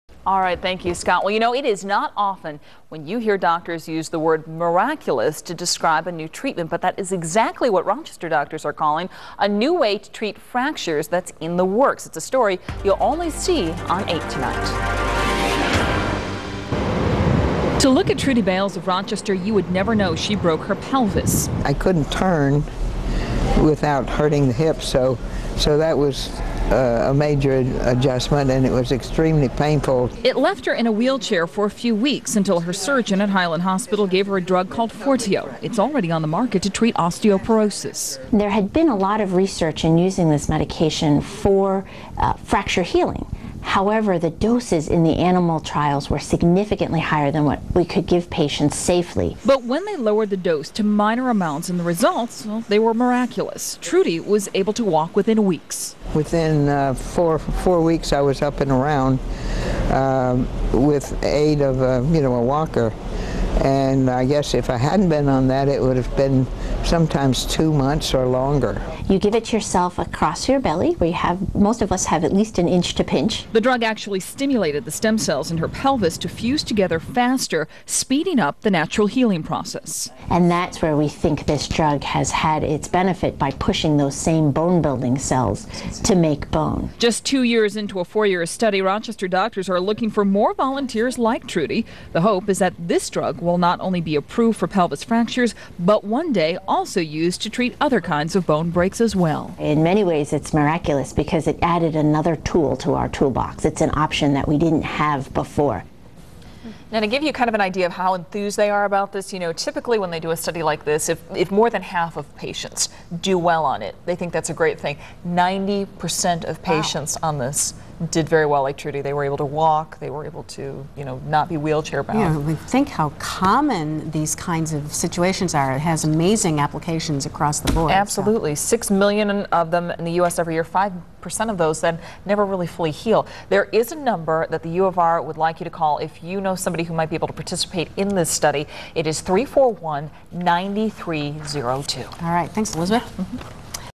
Miraculous Fracture Treatment
Physicians discuss "miraculous" treatment that is helping to heal stubborn bone fractures.